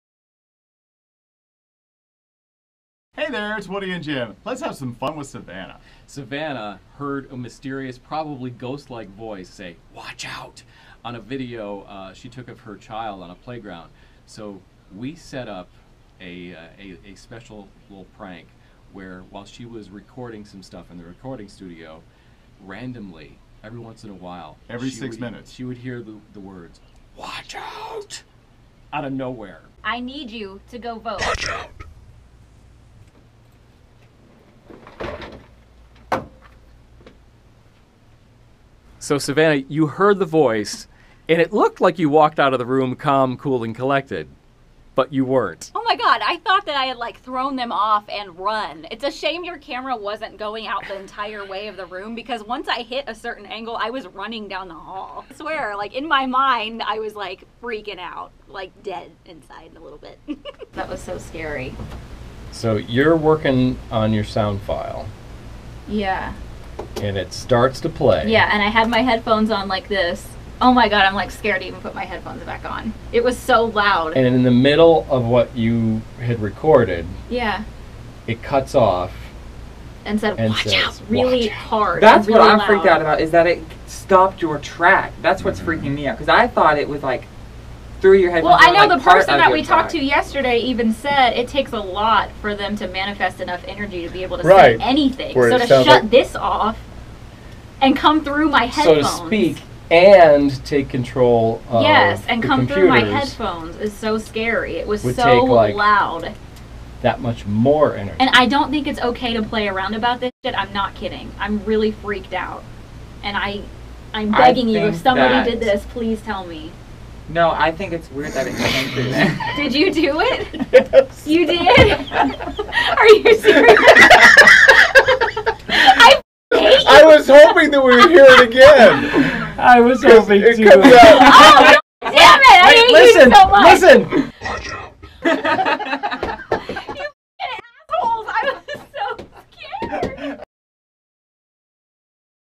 Woody & Jim prank Savannah with demon voice.
She strongly believes in the paranormal. We took advantage of that.